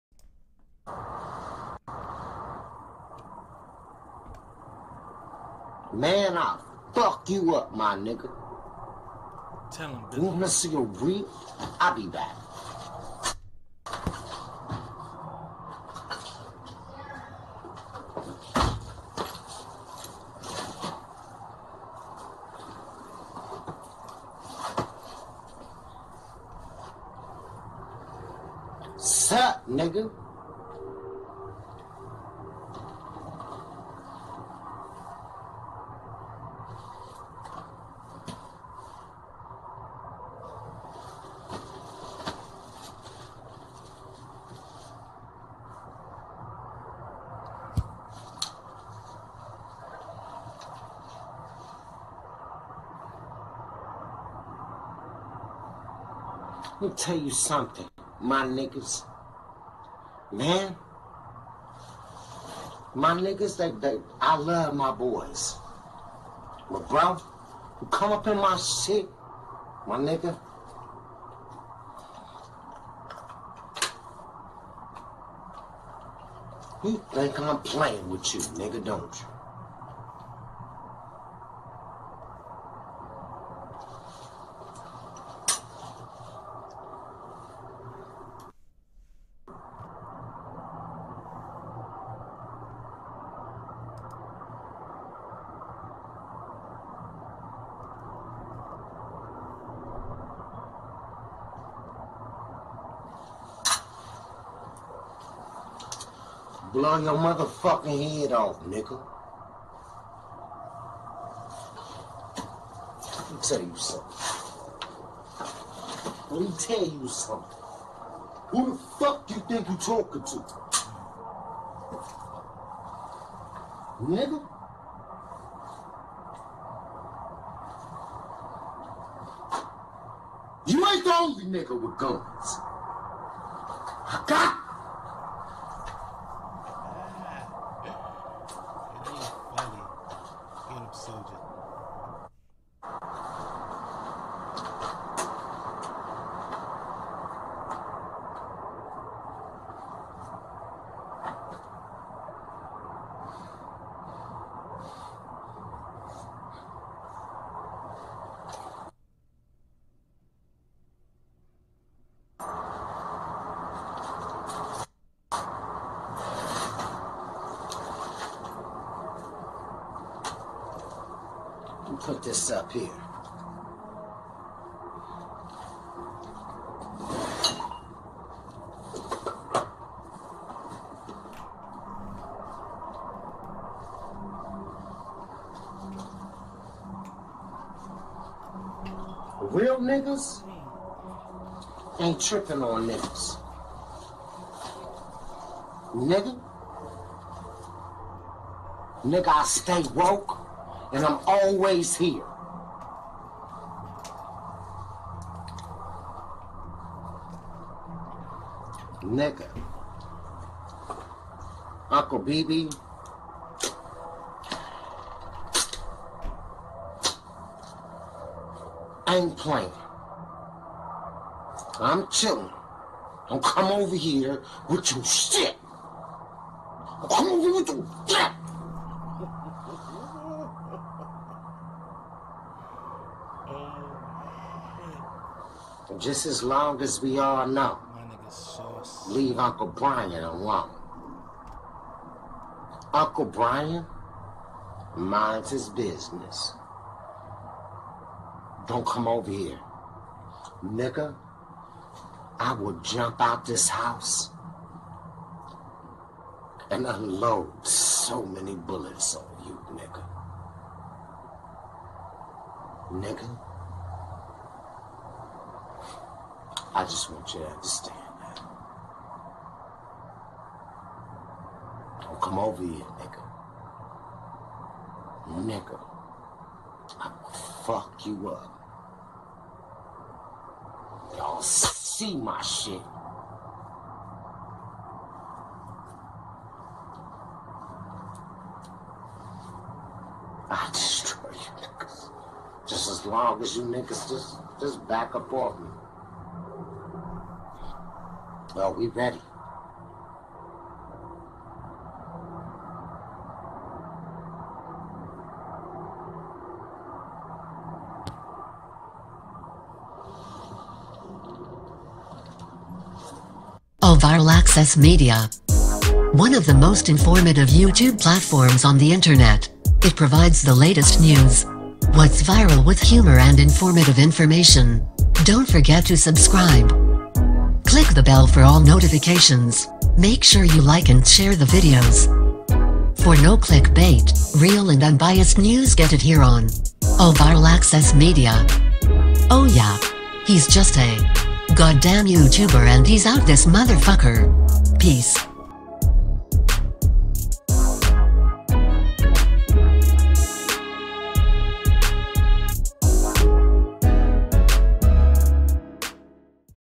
I Would Unload Every Bullet In You Says Bizzy Bone 👀
Taking it from talk to action says bizzy.